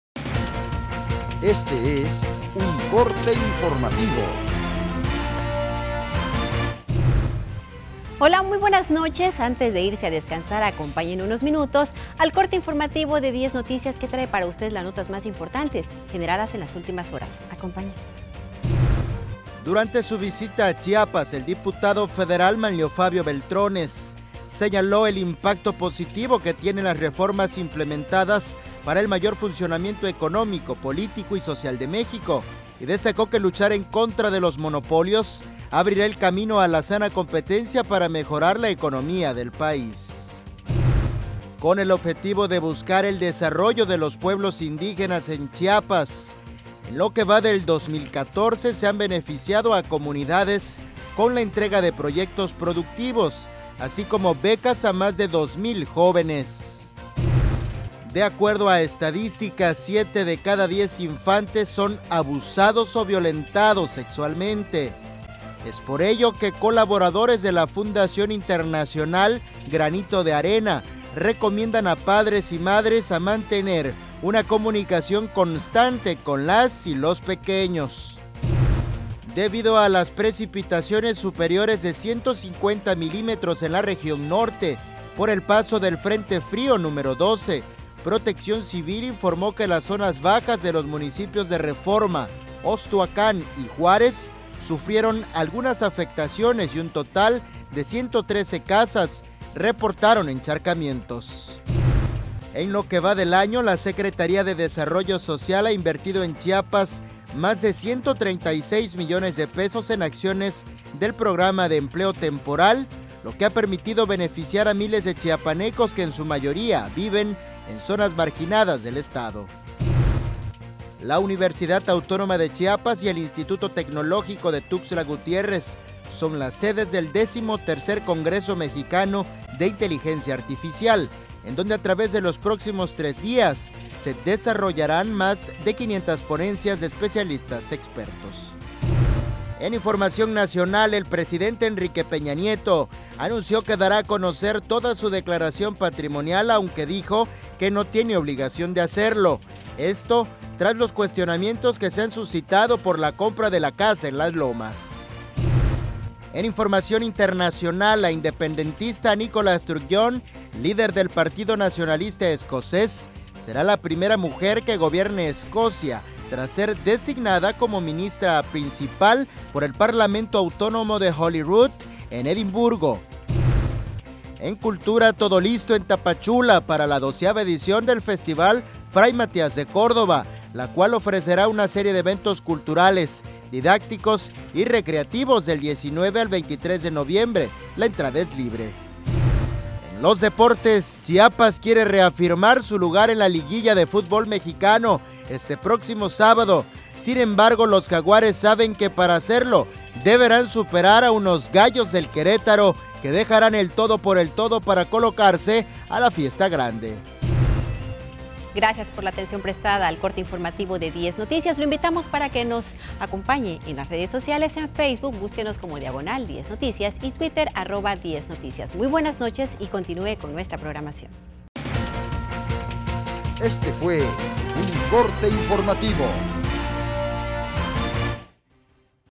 Corte Informativo emisión nocturna (19 de noviembre 2014)
Entérate de las noticias más importantes acontecidas en las últimas horas en el corte informativo de Diez Noticias.